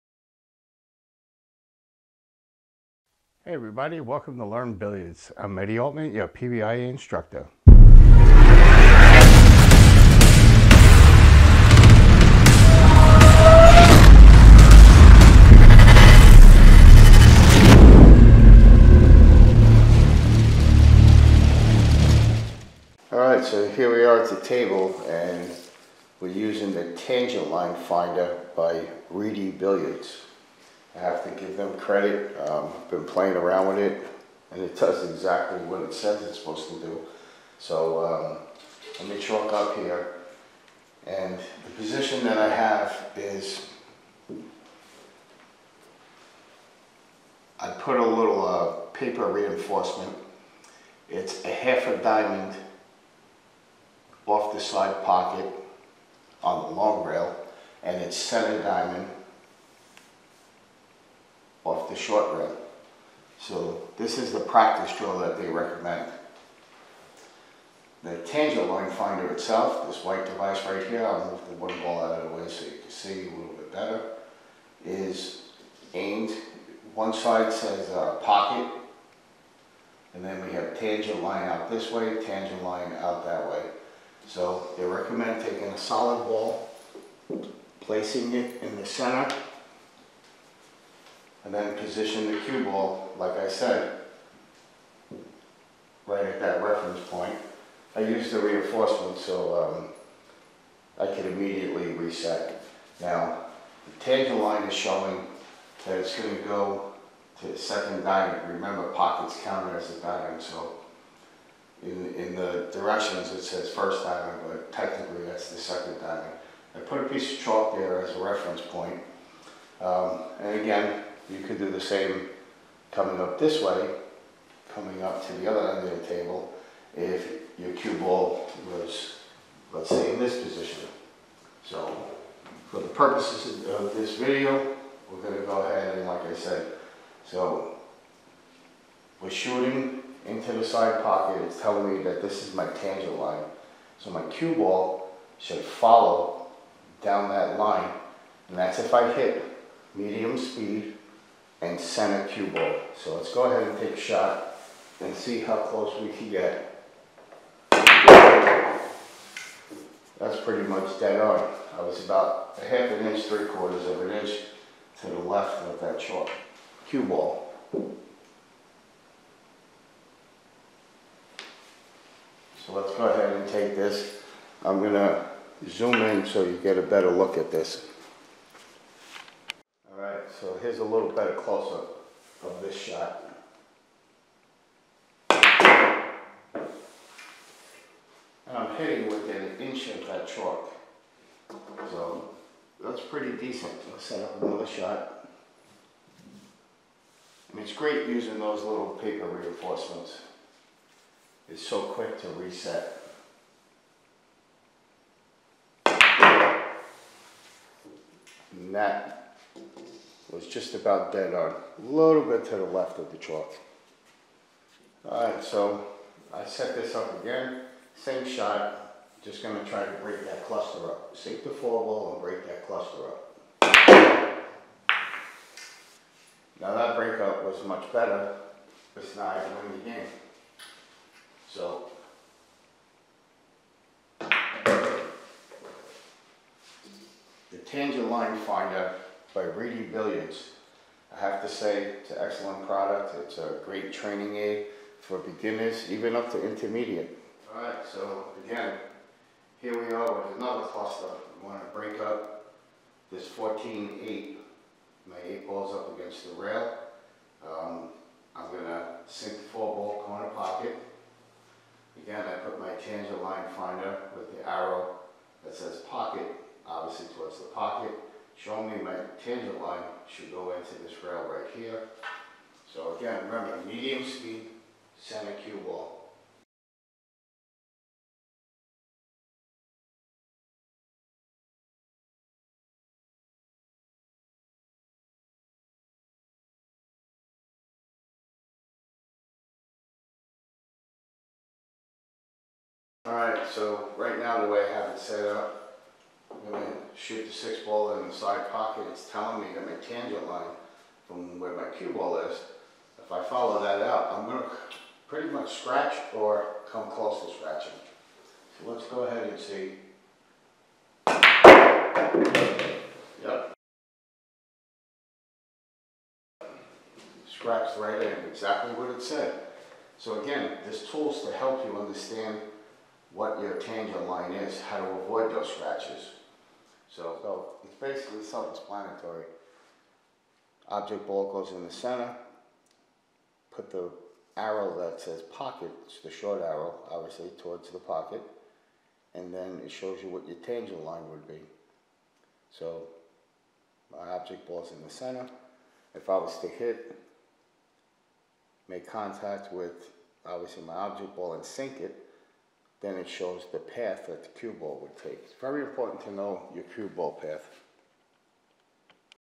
Cue Ball Tangent Line
Cue Ball Tangent Line, find the path of the cue ball with the tangent line finder. Easy to use practice tool to see the tangent line of the cue ball.